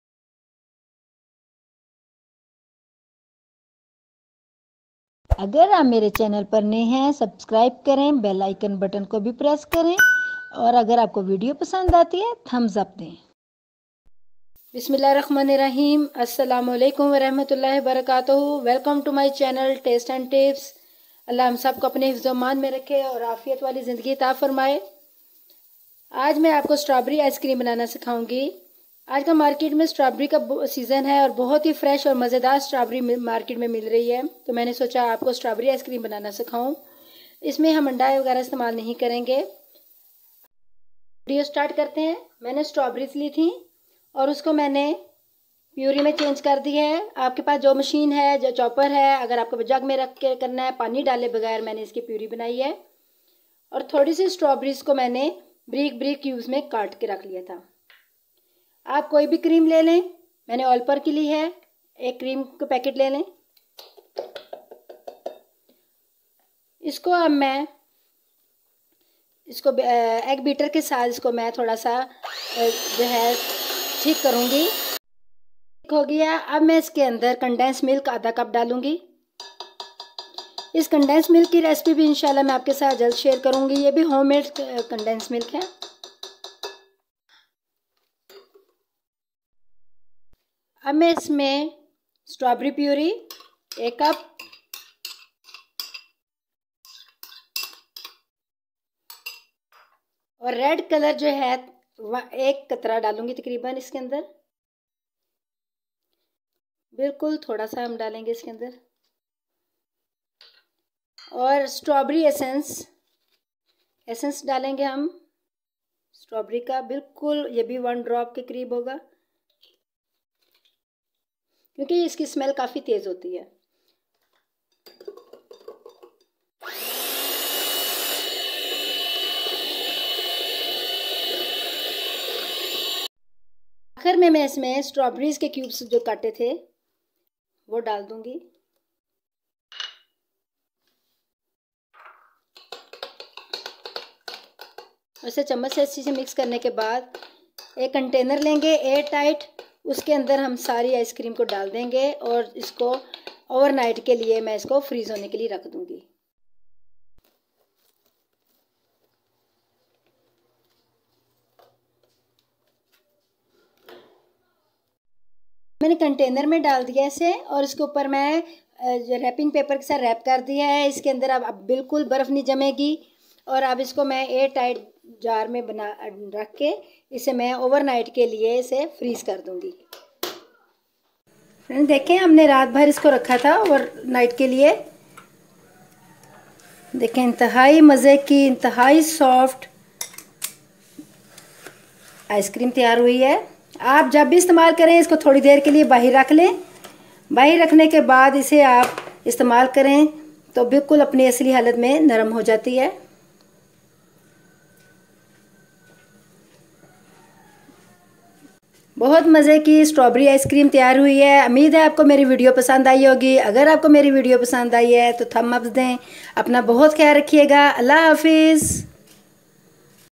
Strawberry ice cream | स्ट्रॉबेरी आइसक्रीम
💯 pour and natural  no chemical and no vegetables fats are use in this recipe enjoy your summer season with it